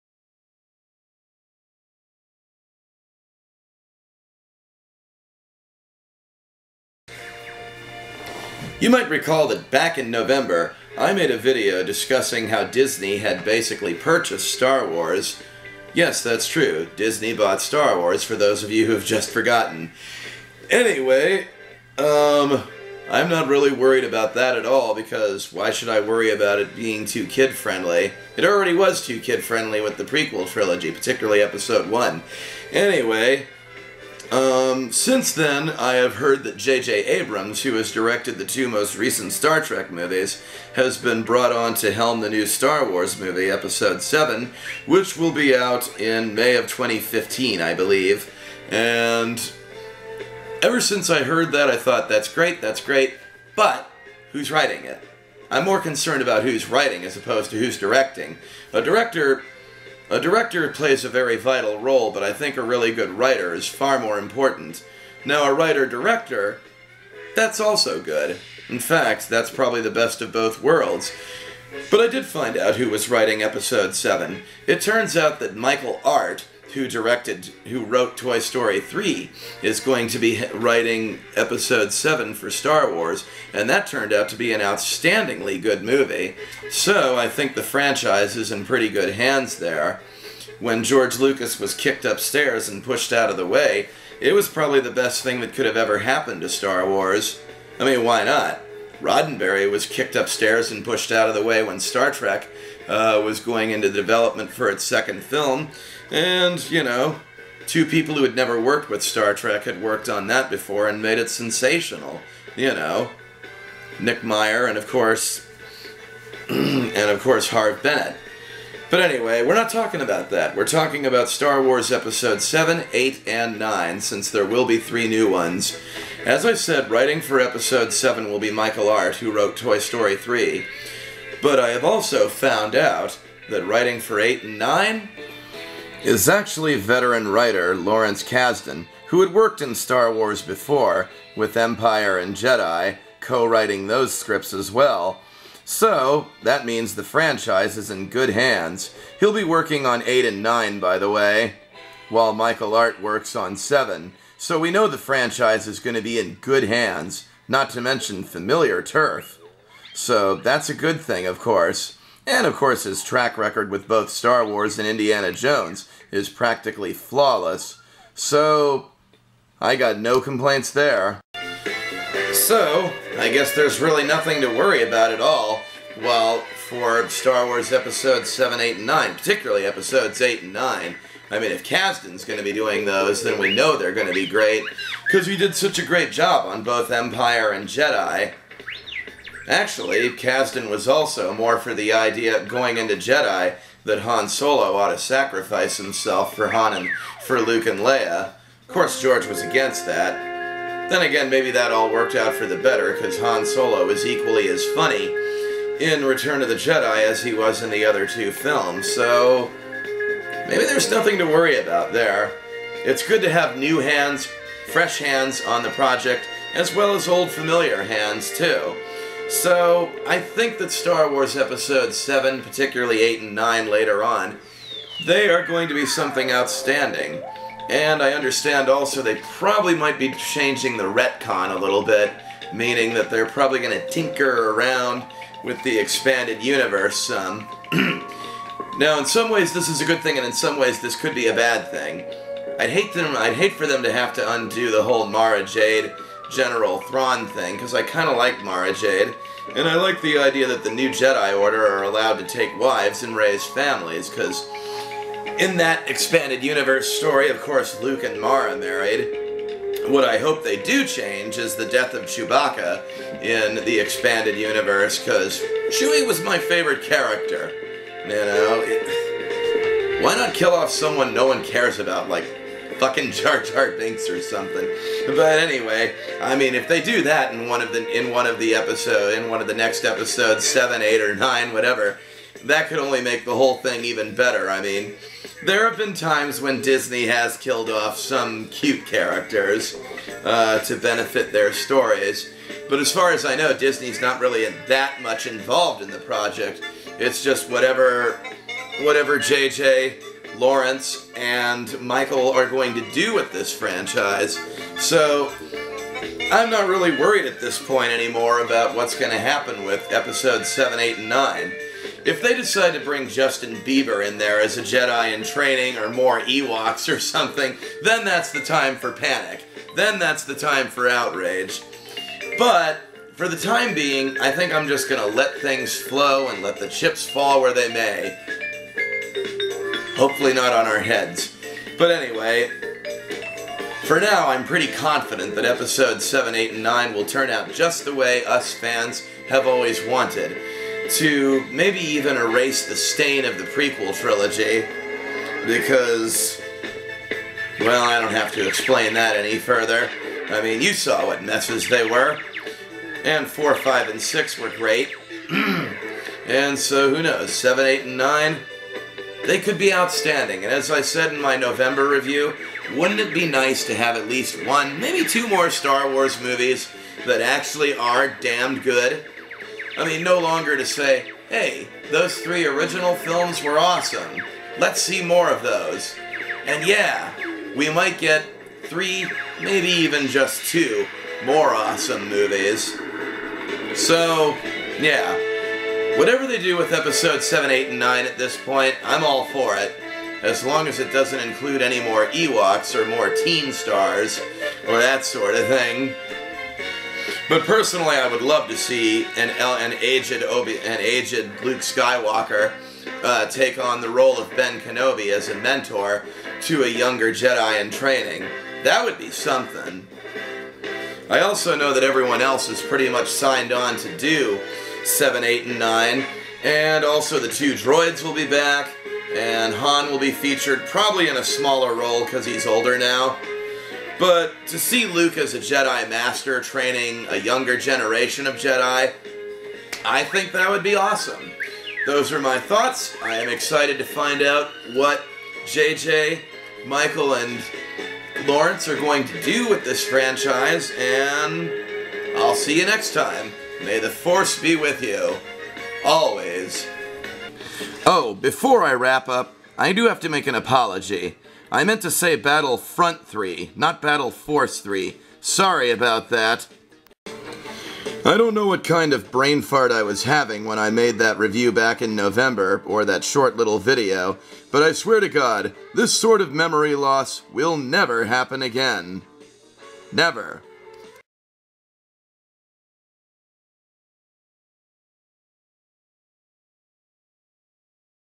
Star Wars VII, VIII & IX: Early Thoughts
I talk about my early thoughts on the next Star Wars Trilogy now that writers have been assigned.